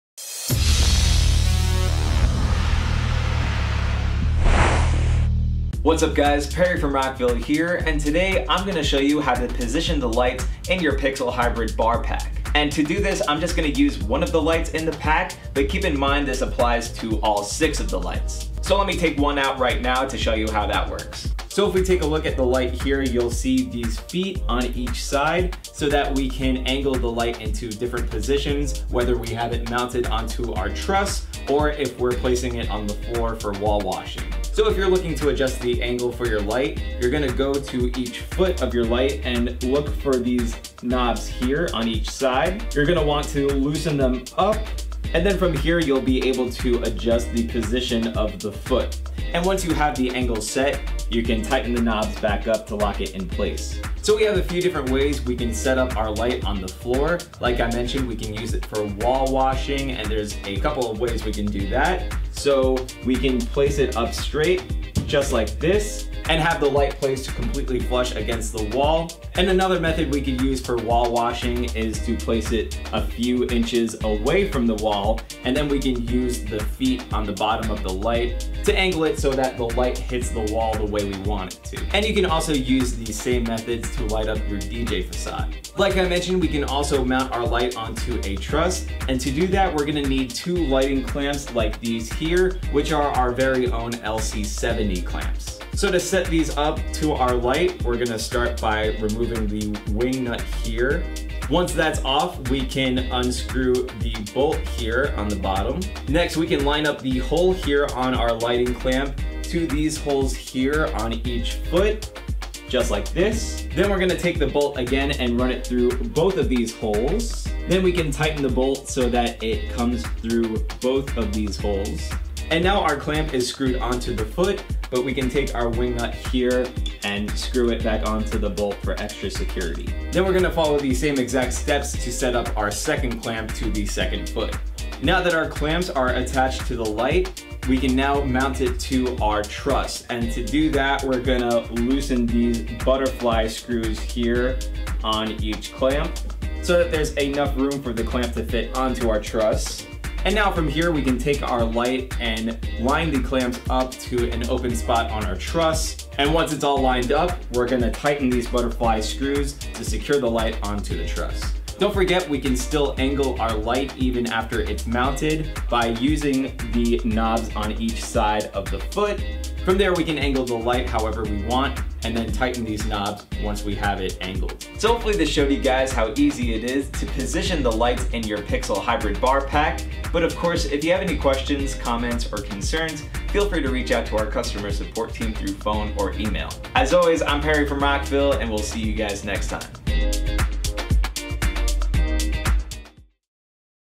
How To Position the Lights w/ Rockville PIXEL HYBRID PACK 6 Battery Wash Light Strips+Charging Case
Applications:
Stage lighting
DJ
Party halls
Bars and Clubs
House Party's

The Rockville PIXEL HYBRID BAR CHARGING CASE is the perfect solution for both protecting your lights during transport and charging your lights efficiently. This case holds 6 PIXEL HYBRID BAR lights. The built in wheels on this case will allow you to safely transport your lights effortlessly.

This durable rolling road case can sure take a beating! We drop tested it with lights inside from 5 feet in the air on both sides and the case along with the lights did not get damaged at all. When you are ready to charge your lights either before or after your gig, simply plug the fixtures into the charging strip within the case and plug the case into the wall. In addition to your lights this case has enough room to store both your DMX and power cables.

Features:

Rockville PIXEL HYBRID BAR CHARGING CASE For (6) PIXEL HYBRID BAR Lights or other similar-sized lights
Built-in top quality wheels come pre-assembled
Easily transports up to 6 lights per case
power cables and remotes can fit inside the case
6 individual internal outlets
Case is stackable
Protects your fixtures during transportation with foam padding
Material: Aluminum frame and Plywood
Inside Protection: High density shockproof foam
Two butterfly locks
Four strong handles
Includes Charging cable
built in power strip for 6 lights
power switch with charging light indicator
wheels with breaks
Wheel diameter: 3"
Fuse: 5A
Dimensions of outer case (inches): 45.67” length x 24.02” width x 12.6” height
Unit Weight: 55.16 Lbs
Important: You must keep the case open when charging so the lights stay cool